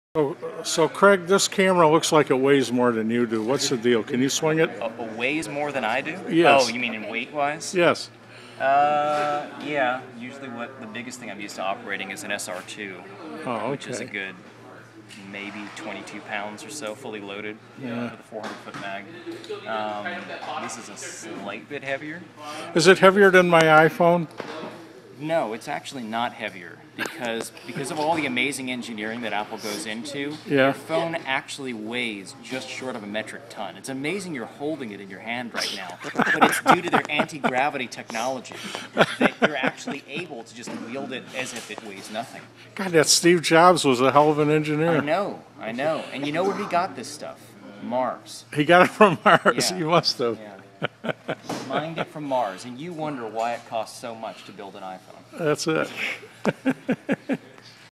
Craig Weinstein at Panavision Checking Out A Camera
Craig explaining the ins and outs of iPhone video technology!